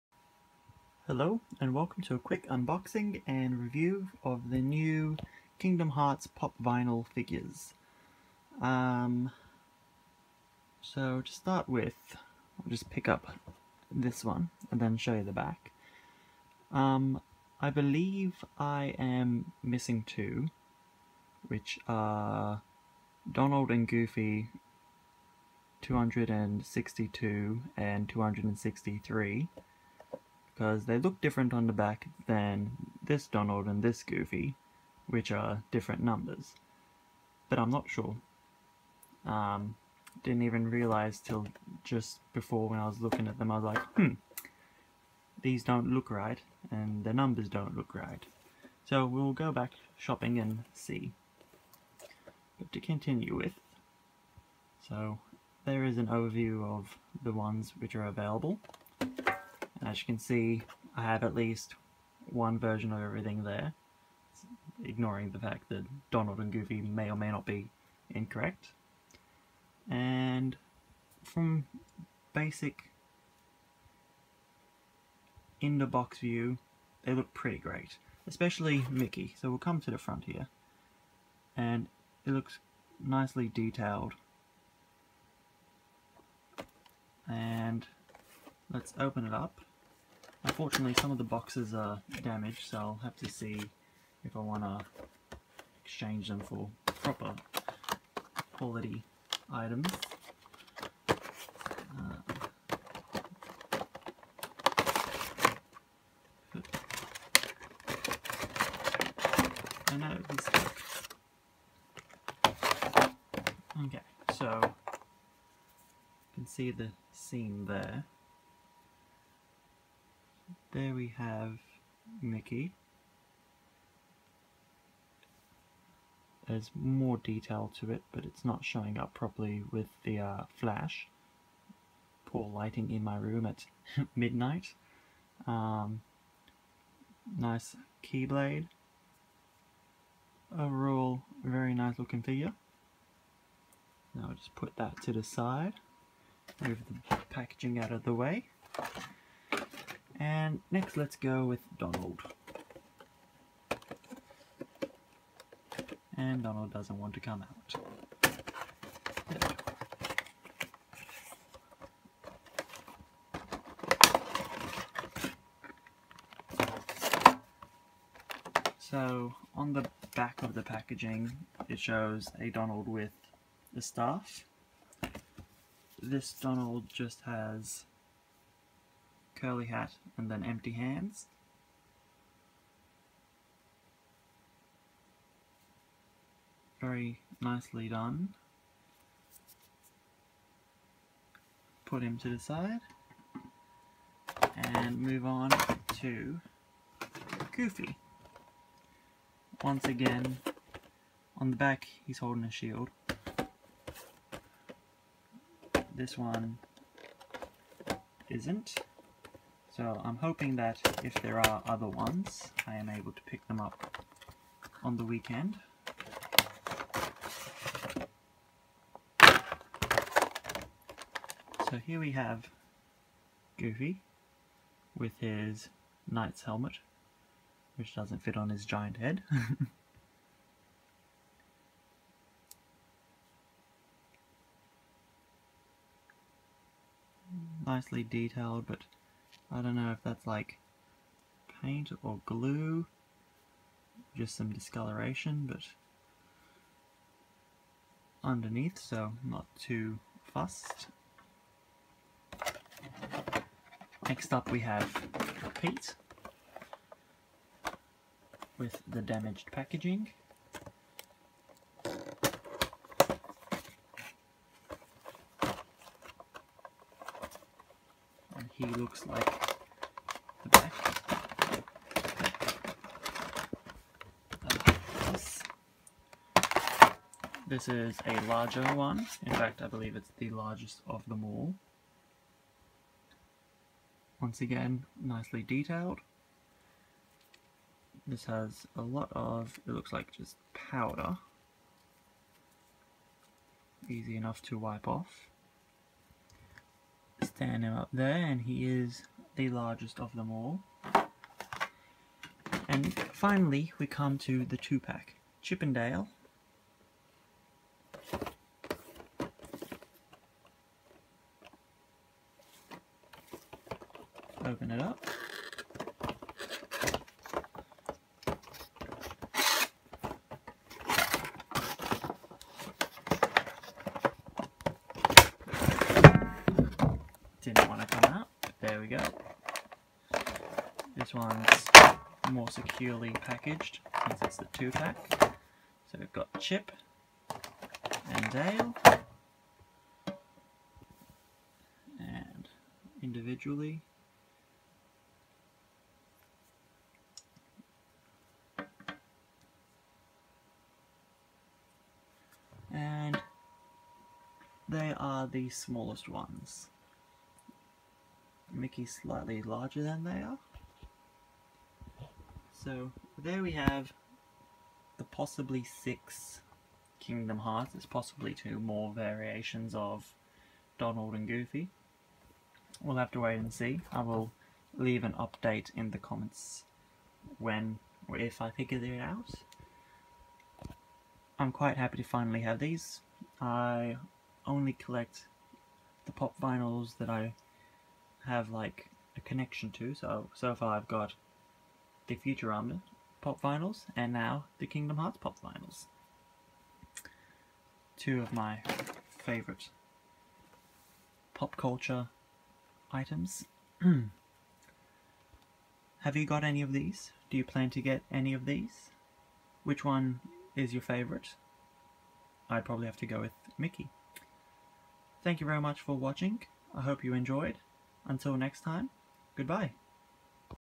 Kingdom Hearts POP! Vinyl Figures Unboxing Review Australia
Quick little Unboxing and Review of the new Kingdom Hearts POP! Vinyl Figures.